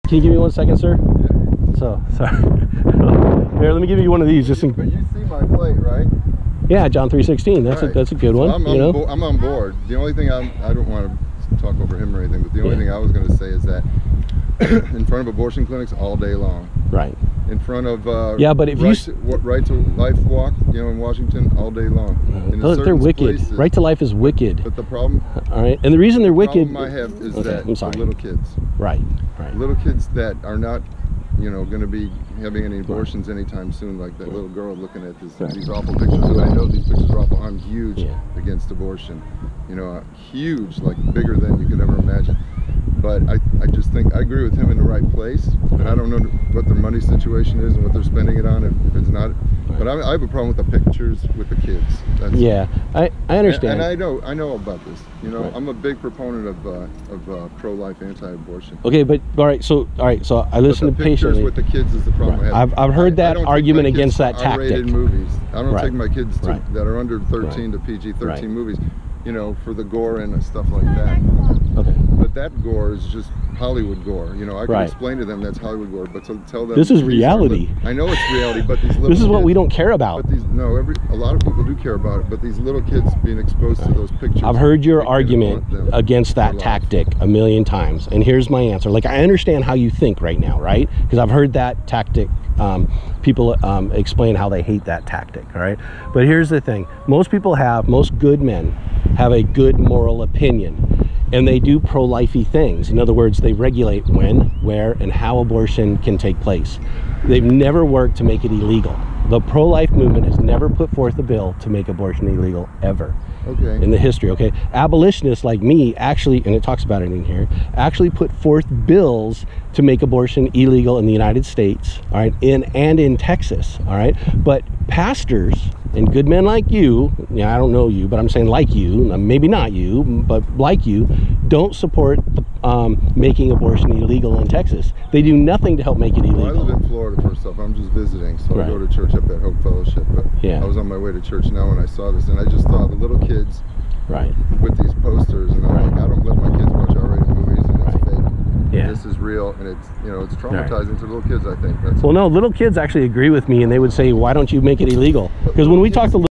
I am On board with you but not the pictures
(Correction Stonebriar Church has a 15 million dollar yearly budget)
Here is a good man who is trying to understand, he lives in florida and stoped to talk to me.